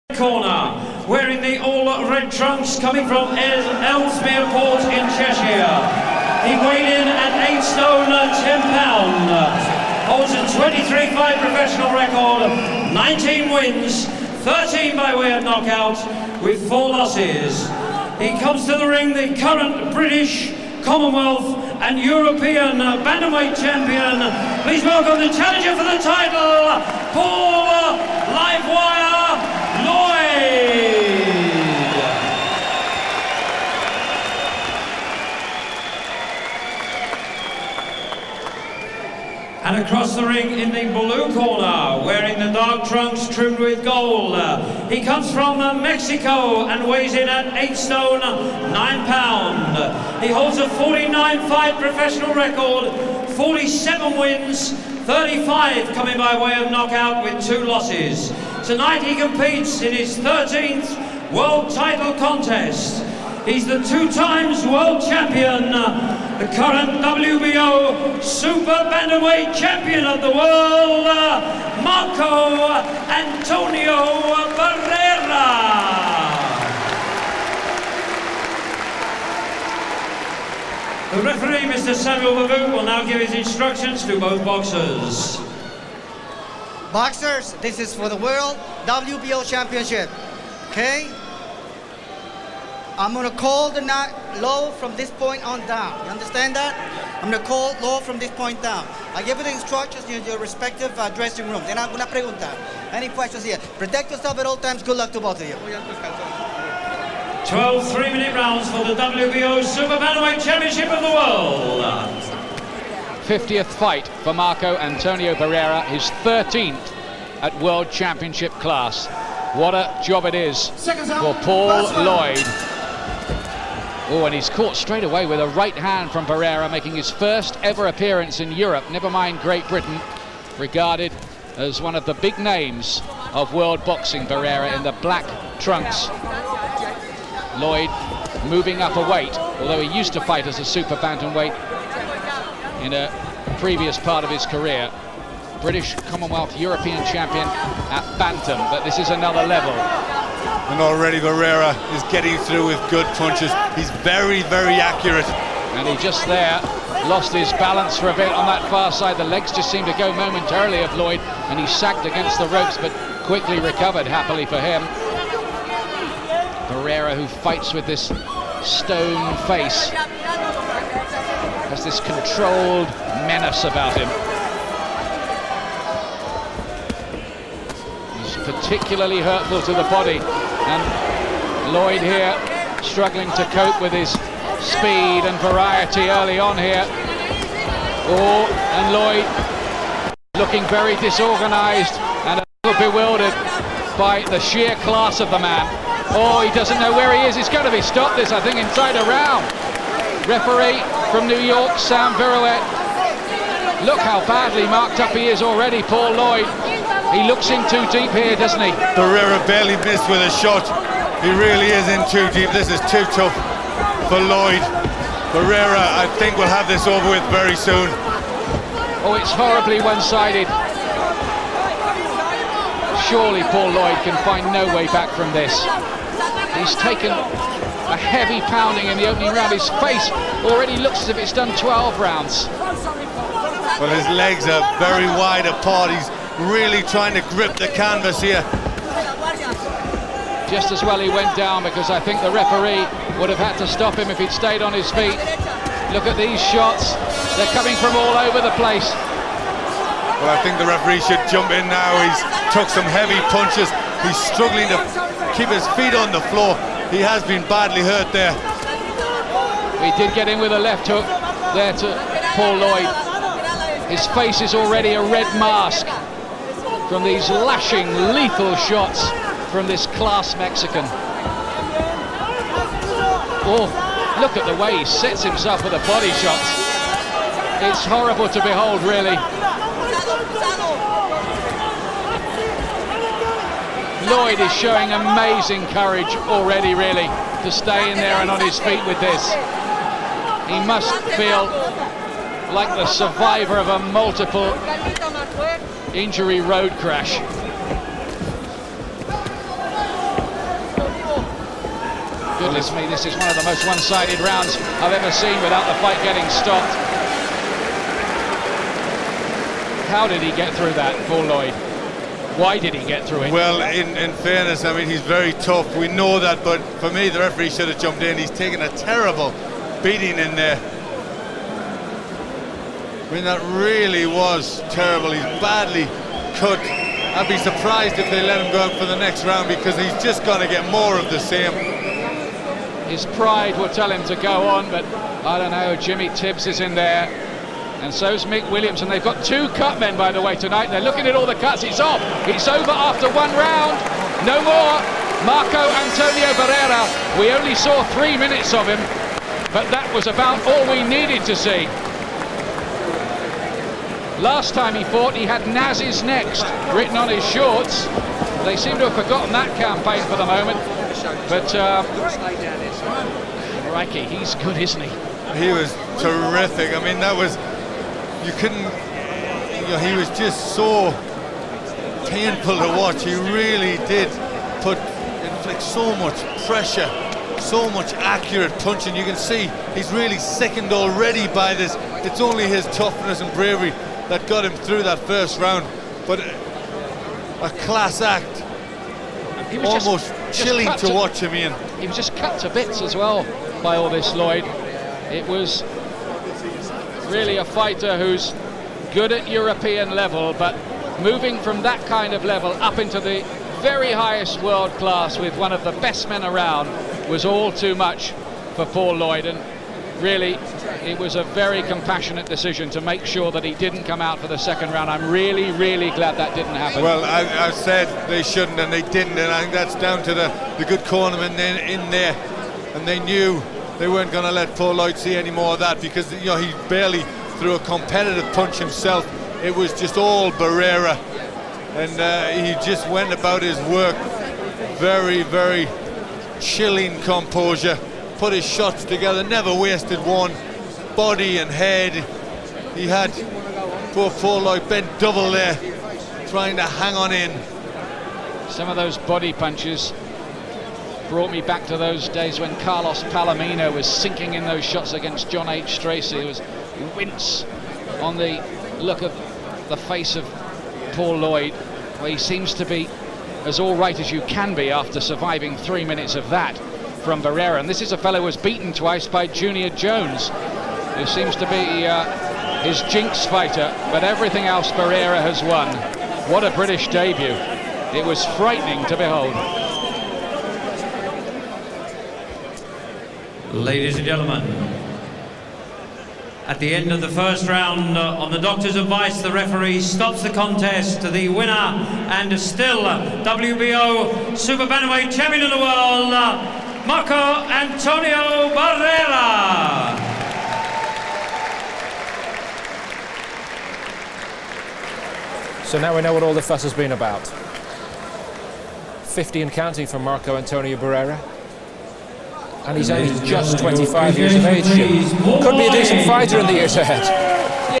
Marco Antonio Barrera vs Paul Llyod
Barrera destroying Britain's Paul Lloyd in his first ever fight in Europe.